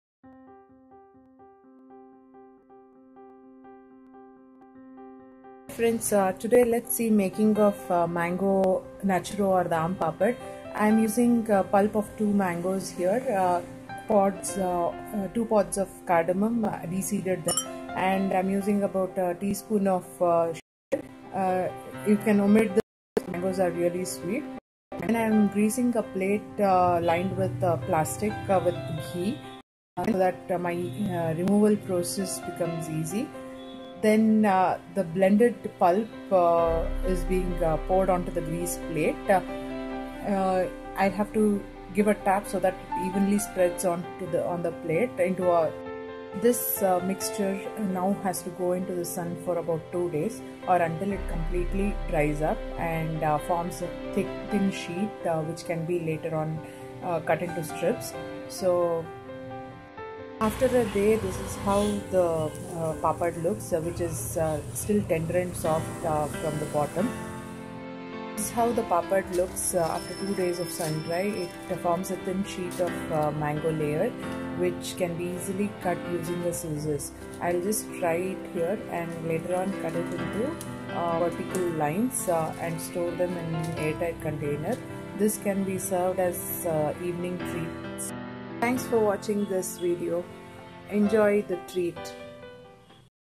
Mango naturo/ Aam papad
A simple treat that you can indulge on without much worry. Its natural and works well when you are carving something sweet.
This can be stored for about a month if precaution taken care to avoid moisture contact.

Do try while mangoes are still around.

Children are bound to love this and will come back for more.